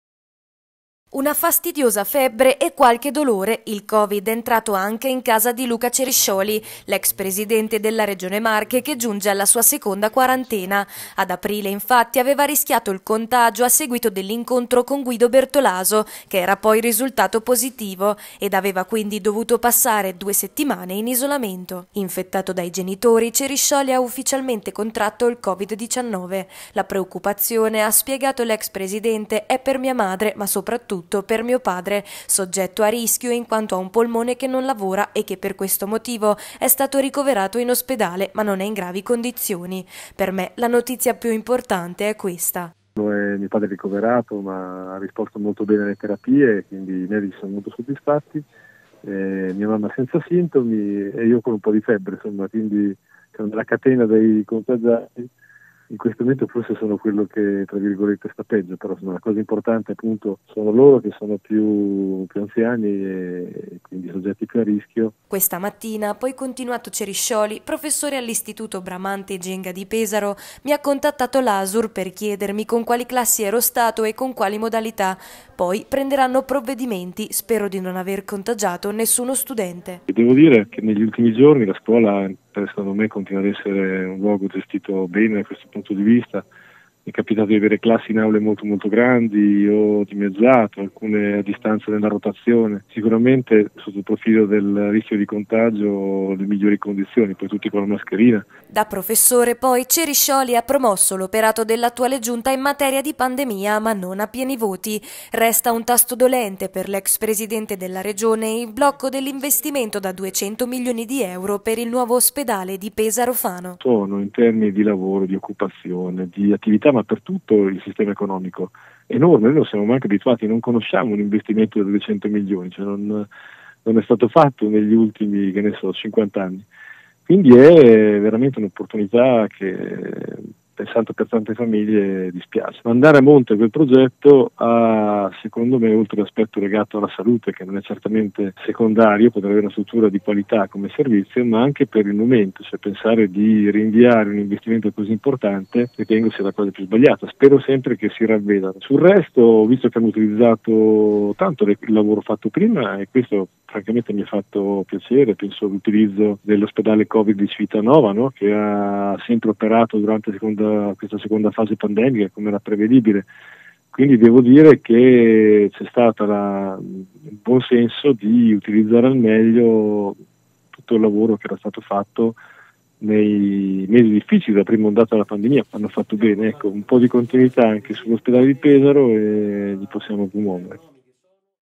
Ceriscioli: "Ho contratto il Covid"
L’ex presidente della Regione Ceriscioli ha contratto il virus a casa dei genitori. Nessuno è in pericolo.